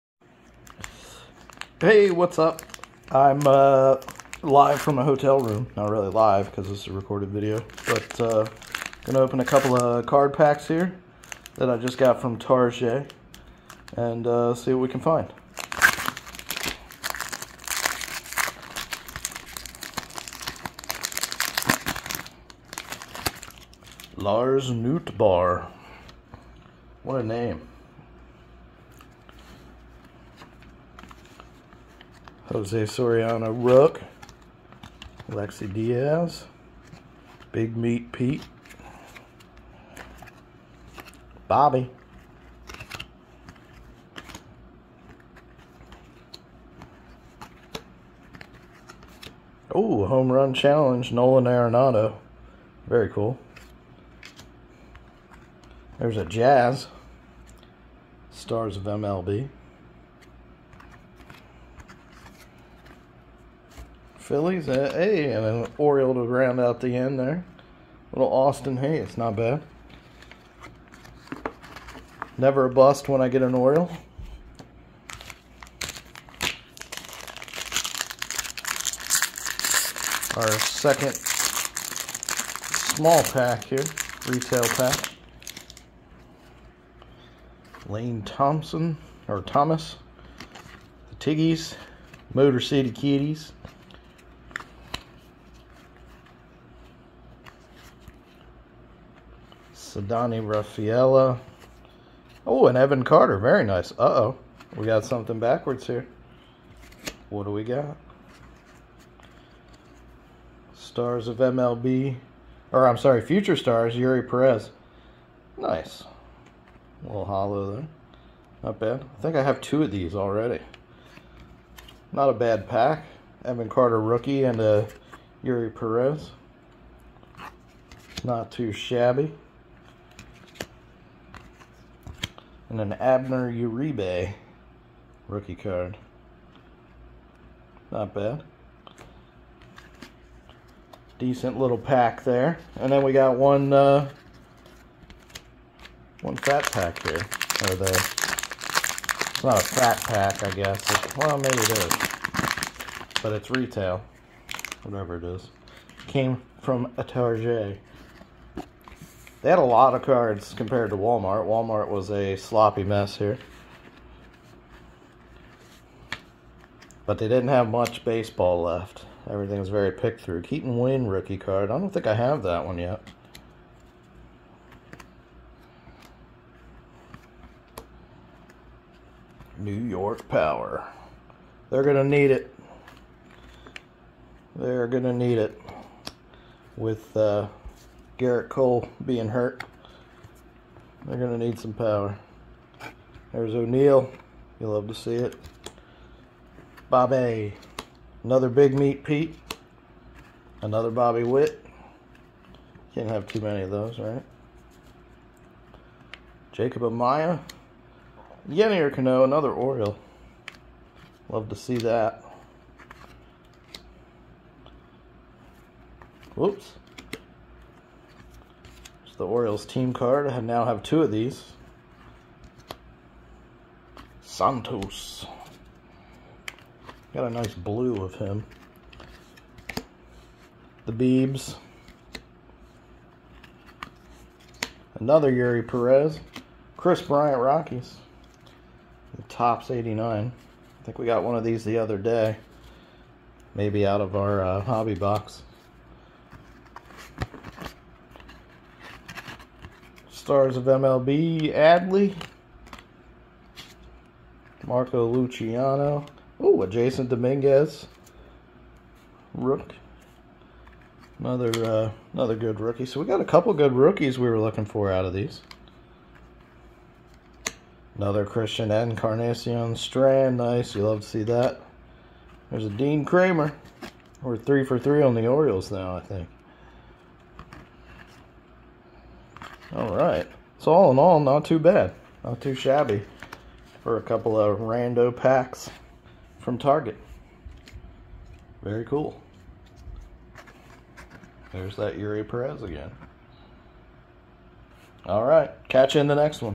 BIG RIPS in my hotel room.
Opening a few Topps 2024 Series 1 packs in my hotel room while I am away for work